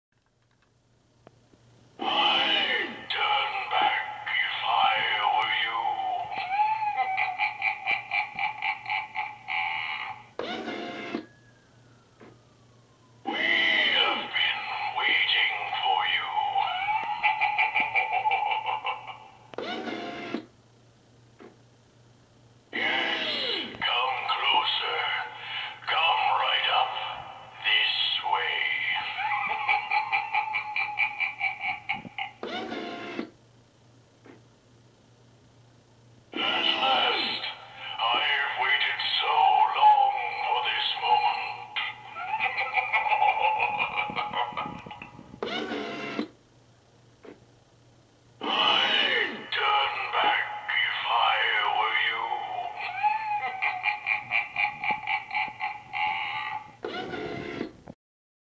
Gemmy Green Face Out Ghost
i orderd this on sale during labor day weekend it is one of the best props this year i really reccomend.enjoy!